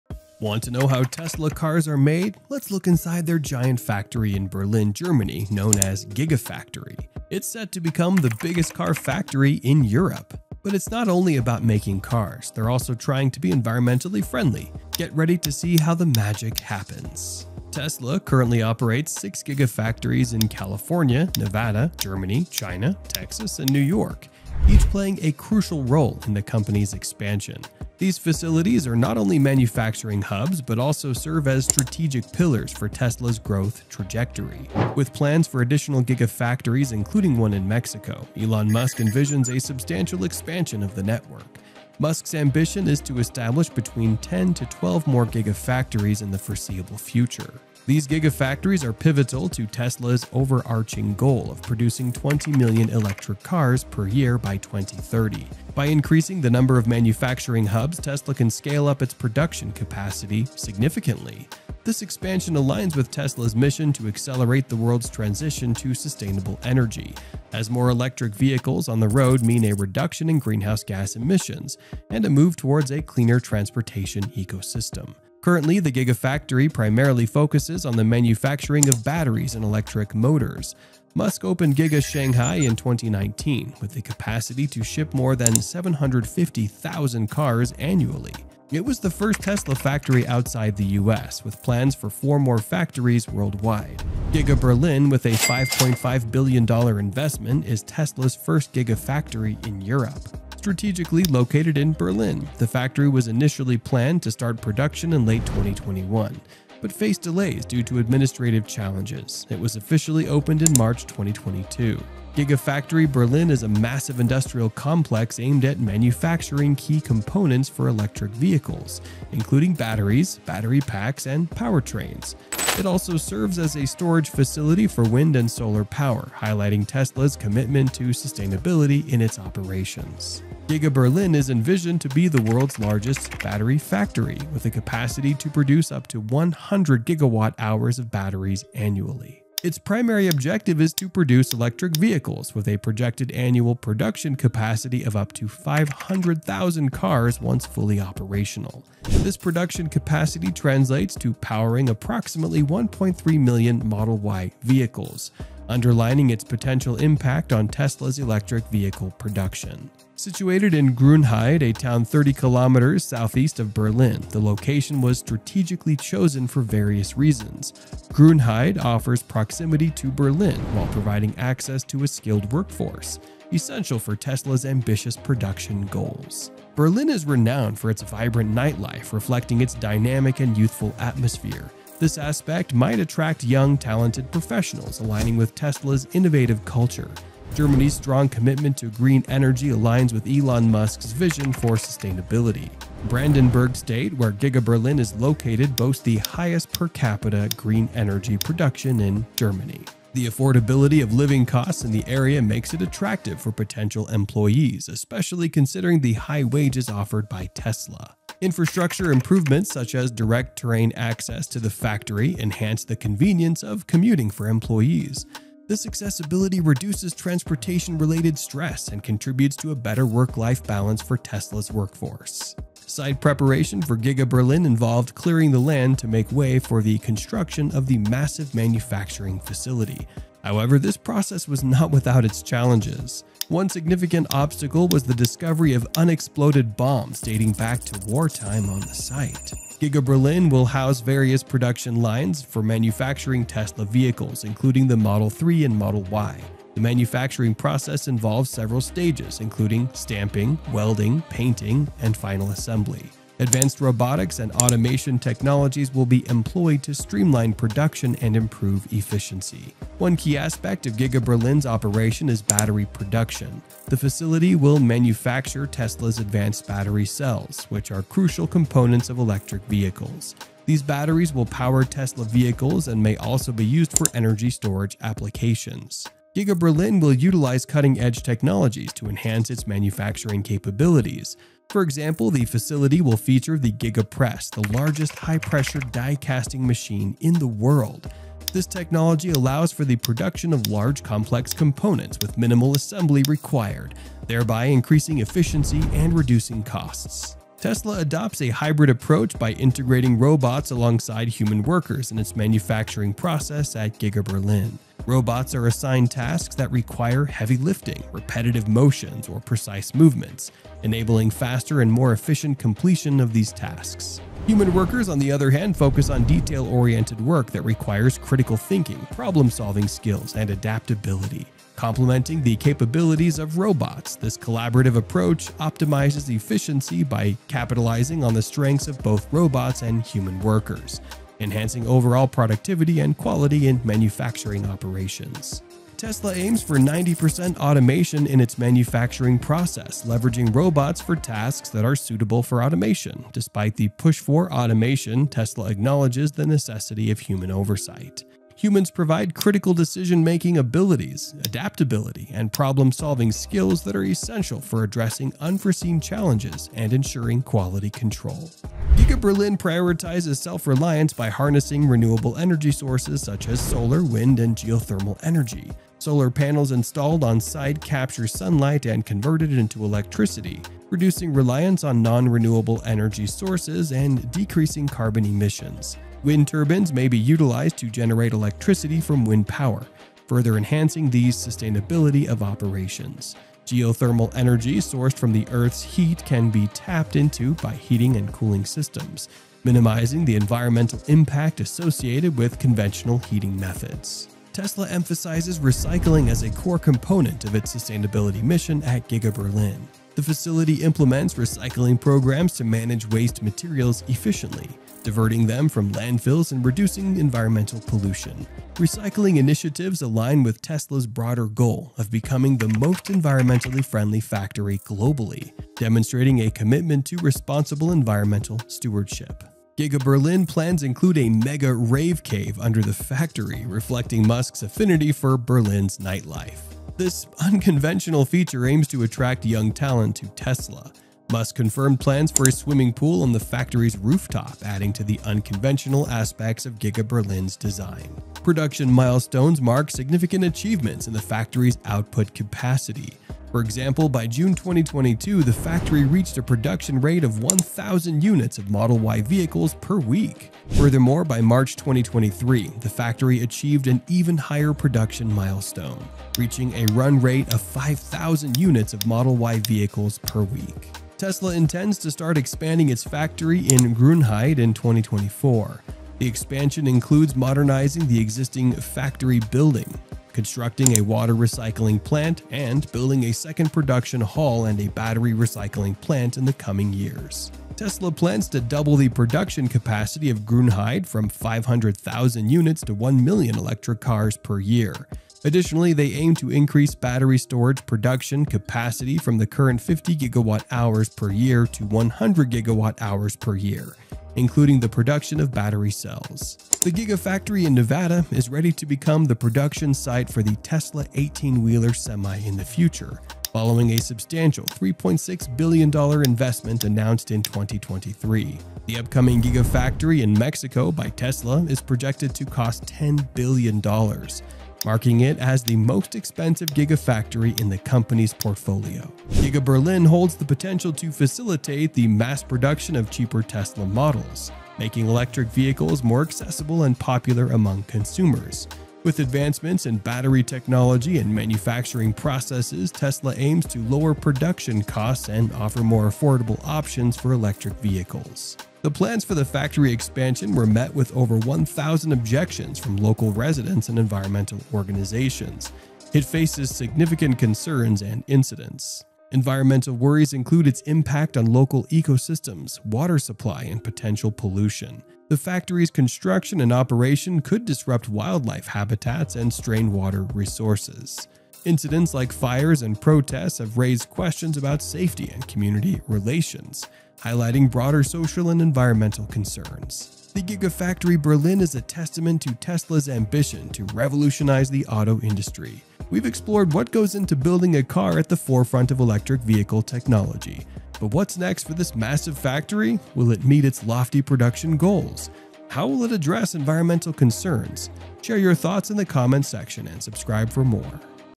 Inside Tesla's Most Advanced Giga factory
🚗 **Inside Tesla's Gigafactory Berlin: How the Future of Cars is Made! 🇩🇪🏭**

Want to know how Tesla cars are made? Join us as we explore Tesla's Gigafactory in Berlin, Germany, set to become Europe's largest car factory. This state-of-the-art facility focuses on producing key components like batteries and electric motors, with a goal of manufacturing up to 500,000 cars annually, including the Model 3 and Model Y. Beyond car production, Gigafactory Berlin emphasizes sustainability with solar panels, wind turbines, and recycling initiatives, aligning with Tesla's mission to accelerate the world's transition to sustainable energy. With ambitious plans for expansion, including a projected output of 20 million electric cars per year by 2030, Tesla is revolutionizing the automotive industry one Gigafactory at a time. 🌟🔋🚘